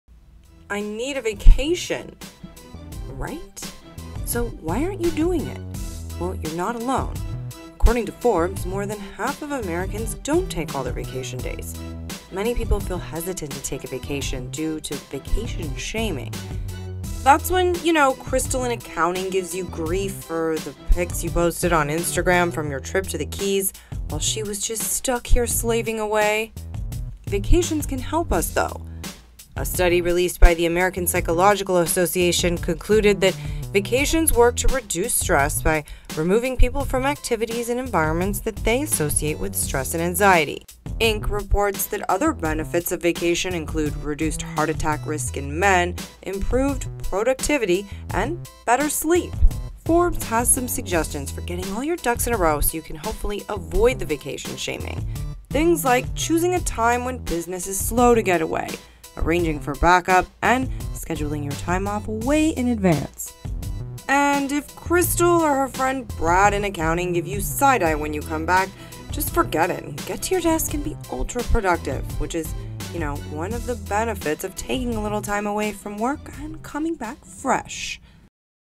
Stop Vacation Shaming and Start Vacation Claiming
Buzz60's Elizabeth Keatinge tells us why you should probably take a vacation.